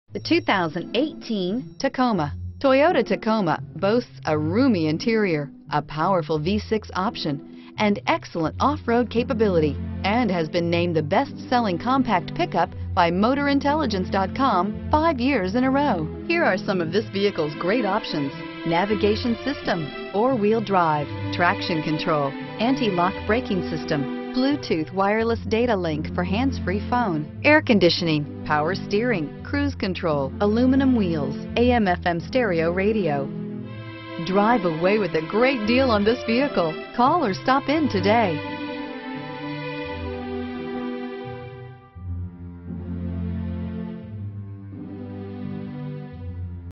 2018 Toyota Tacoma Live Video! Tampa, Wesley Chapel, Brandon, New Port Richey, FL Live 184554
2018 Toyota Tacoma TRD Off Road

Stadium Toyota
5088 N. Dale Mabry Highway

Unleash your sense of adventure in our 2018 Toyota Tacoma TRD Off-Road Double Cab 4WD that looks bold in Magnetic Gray Metallic! Powered by a 3.5 Liter V6 that offers 278hp while matched to a 6 Speed Automatic transmission for amazing road authority. Endowed with multi-terrain select, crawl control, and an electronically locking rear differential, our brawny Four Wheel Drive truck helps you tame the toughest trails. Conquer your day in our Tacoma TRD Off-Road that grabs attention with its muscular fenders, distinct grille, and alloy wheels.     The Tacoma cabin is ready for action and helps you stay comfortable and relaxed with push-button start, a multi-information display, and power windows/locks. Maintain a connection thanks to Entune Audio that features a touchscreen display, impressive audio, voice recognition, wireless smartphone charging, Bluetooth, and more!     Named the best resale value by KBB, our Tacoma is equipped with the Star Safety System, advanced airbags, and a backup camera to offer you additional peace of mind. Our Tacoma is primed to exceed your demands and is built for fun.